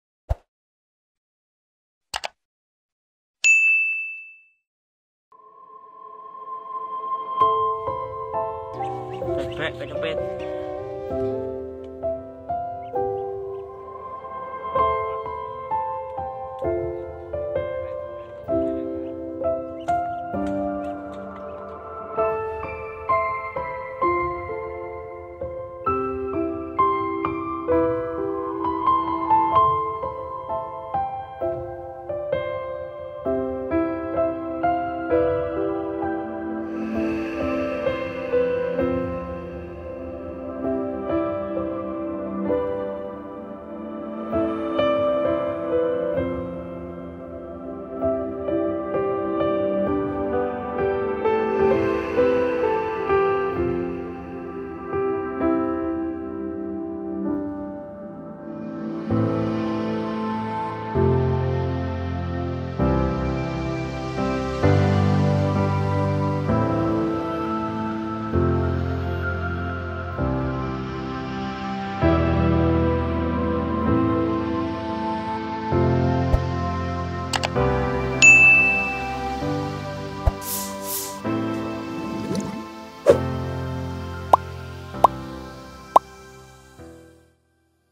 Seekor Bebek Terjebak Abu Vulkanik Semeru, Relawan Bergegas Menyelamatkan
Sebuah video memperlihatkan seekor bebek yang terjebak di abu vulkanik gunung semeru.

Dalam video terlihat kaki bebek terkubur abu vulkanik saat erupsi terjadi.

Hal ini terekam dalam video yang diunggah akun TikTok @beatyom.

Video terkini dan terbaru..